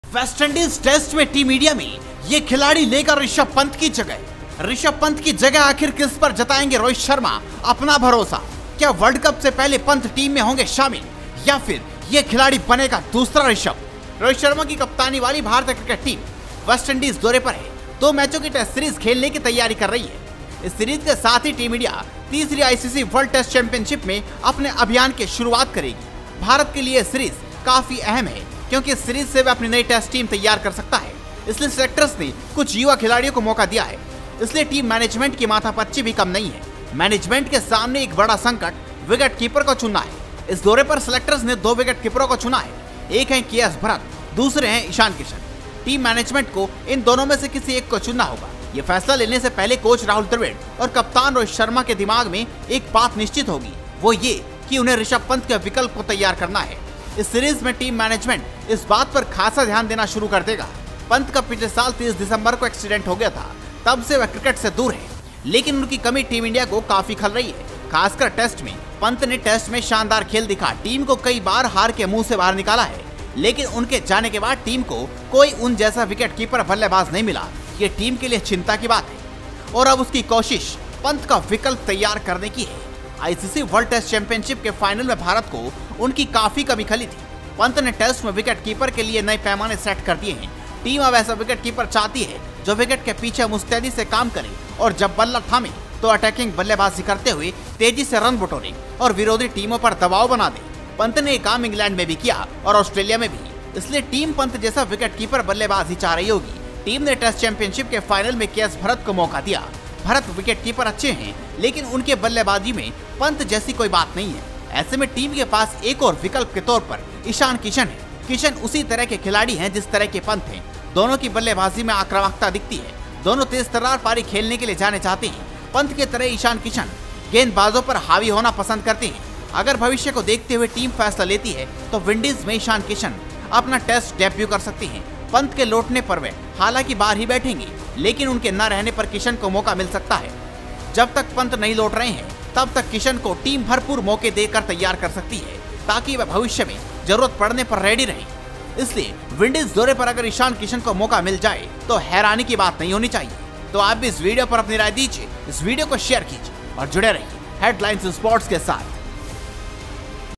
वेस्टइंडीज टेस्ट में टीम इंडिया में ये खिलाड़ी लेगा ऋषभ पंत की जगह ! | Headlines Sports
वेस्टइंडीज टेस्ट में टीम इंडिया में ये खिलाड़ी लेगा ऋषभ पंत की जगह !